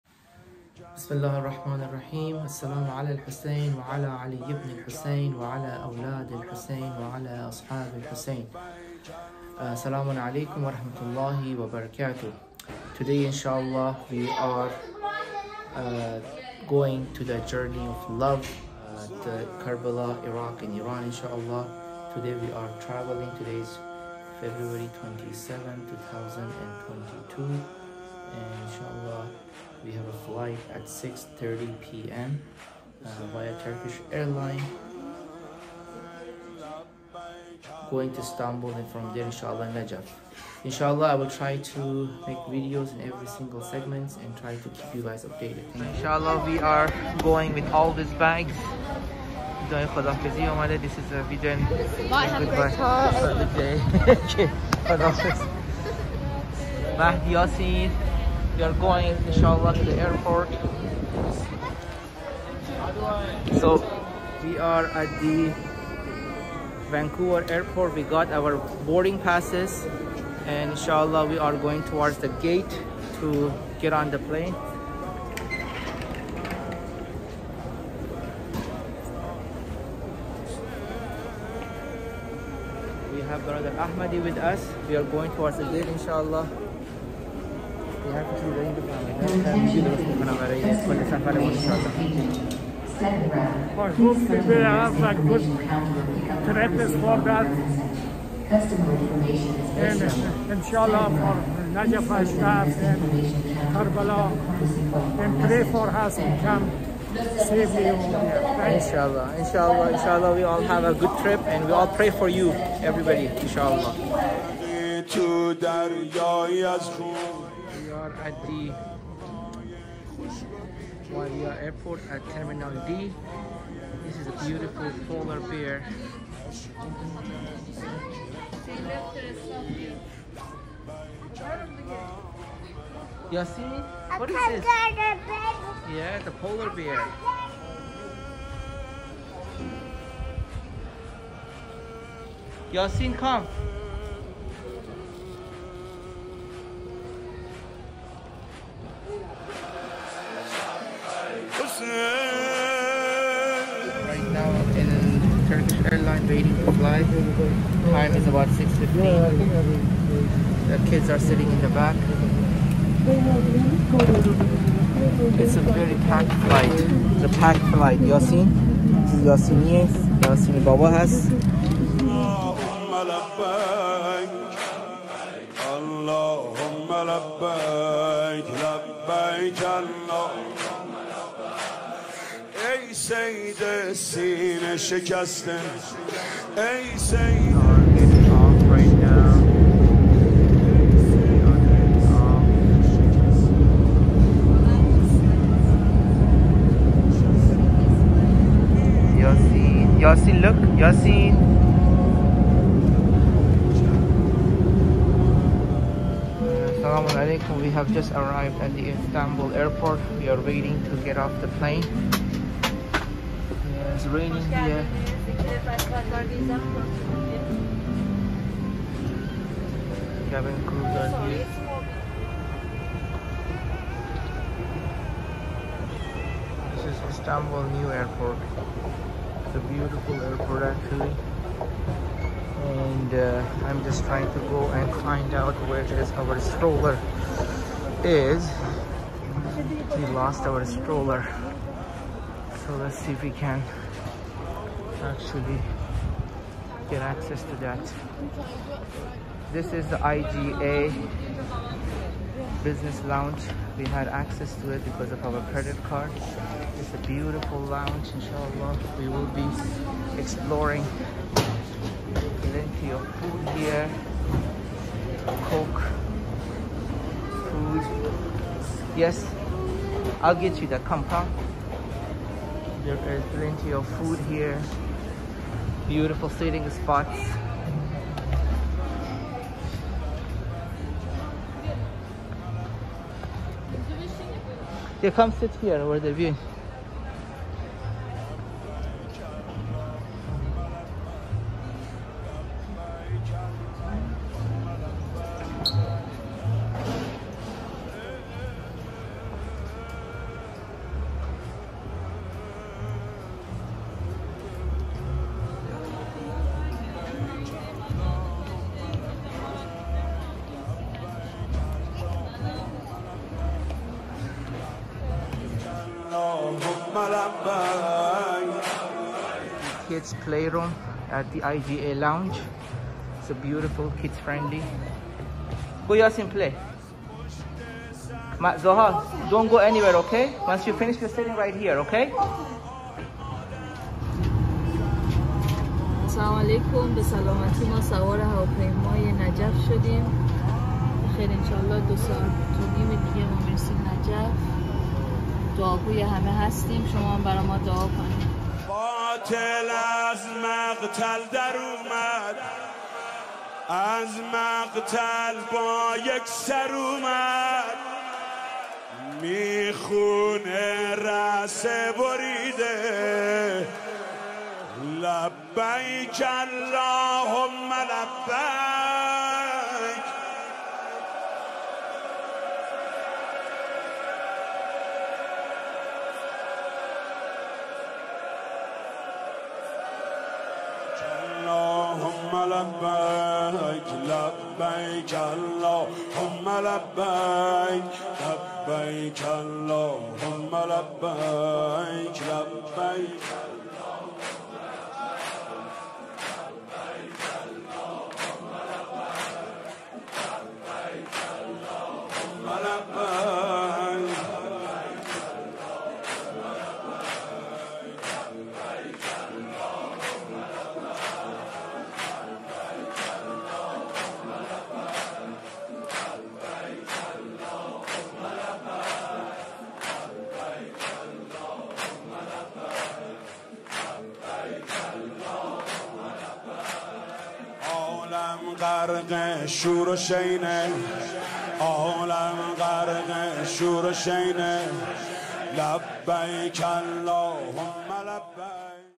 Iraq and Iran trip part 1
Family trip of pilgrimage to Iraq and Iran

Background track is by Ostad Mahmood Karimi